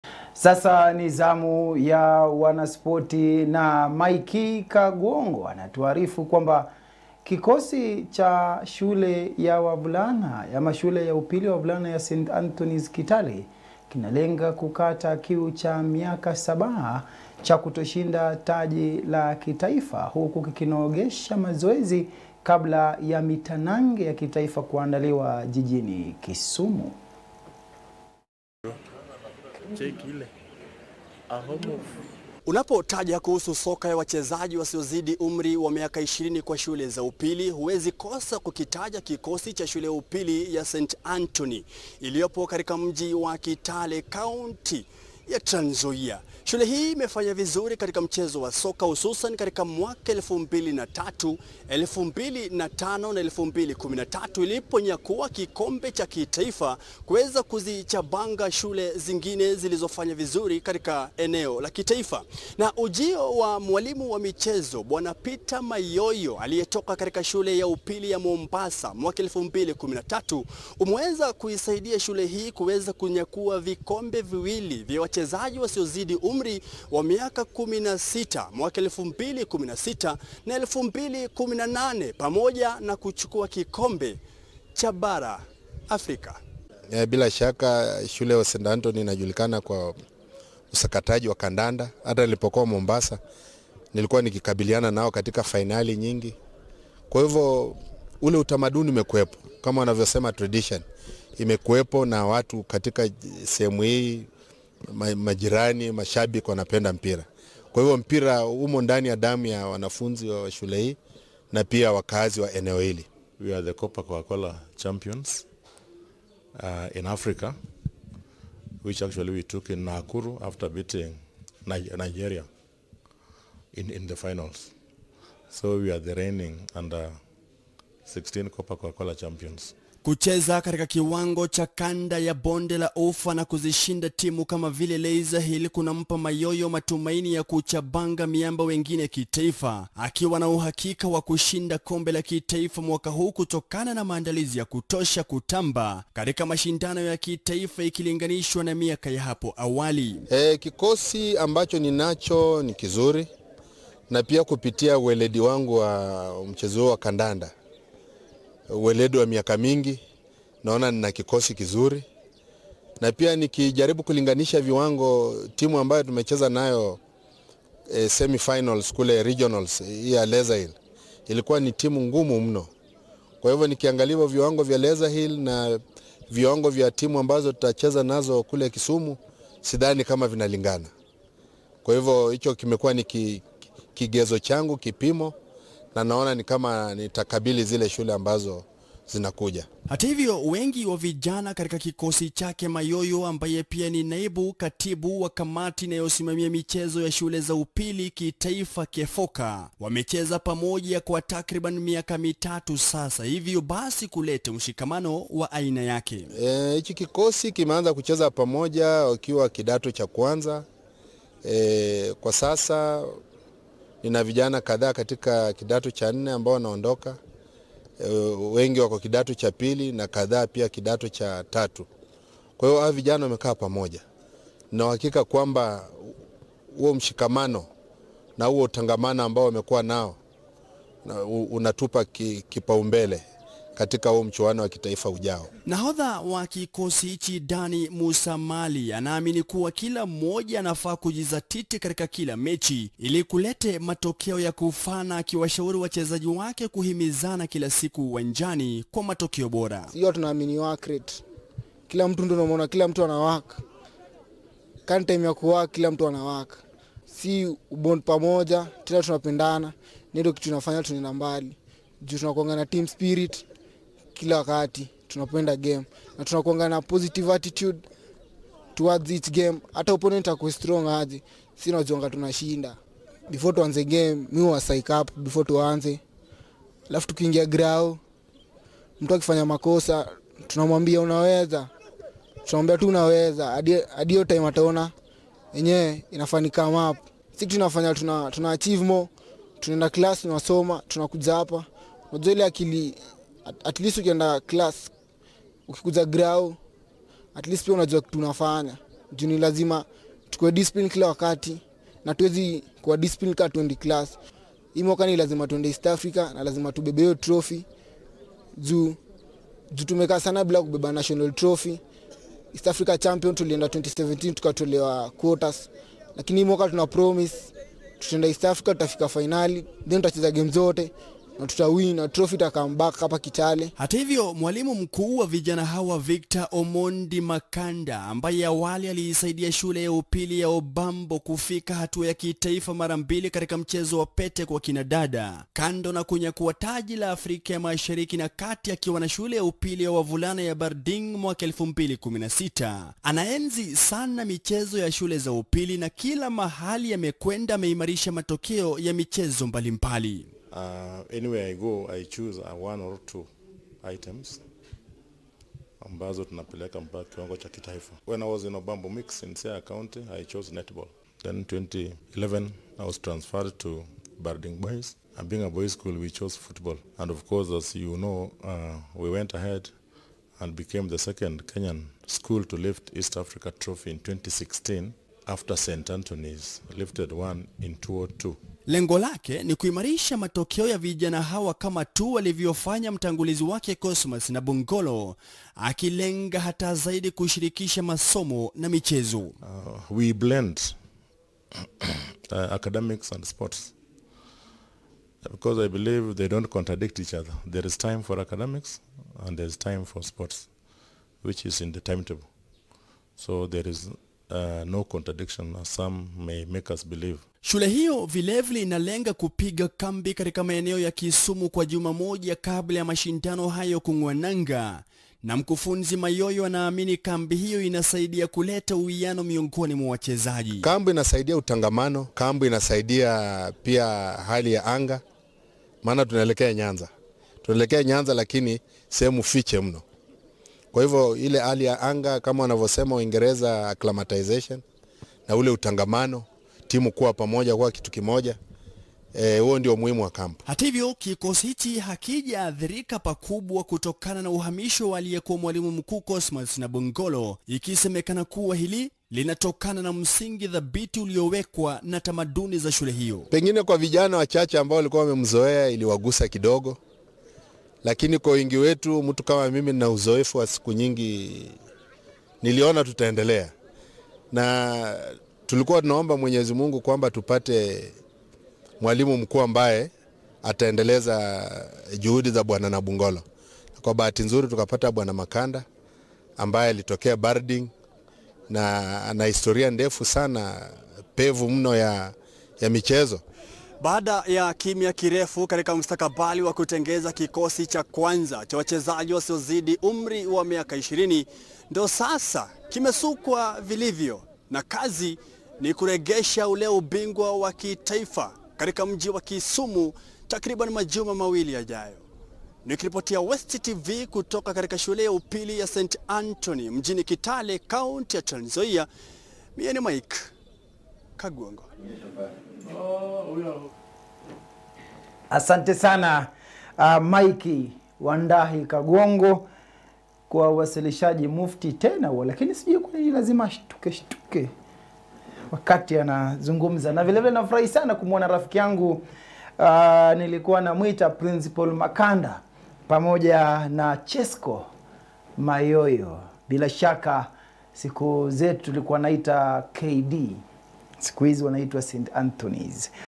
KIKOSI CHA ST ANTHONY'S BOYS KINALENGA UBINGWA KITAIFA BAADA YA UKAME WA MIAKA SABA
Kikosi Cha Shule Ya Upili Ya St. Anthony's Kitale Kinalenga Kukata Kiu Cha Miaka Saba Ya Kutoshinda Taji La Kitaifa Kwa Soka Ya Wachezaji Wasiozidi Umri Wa Miaka 20  Huku Kikinogesha Mazoezi Mbele Ya Mashindano Ya Kitaifa Kwa Shule Za Upili Katika Jiji La Kisumu
Kaguongo Mike anaeleza zaidi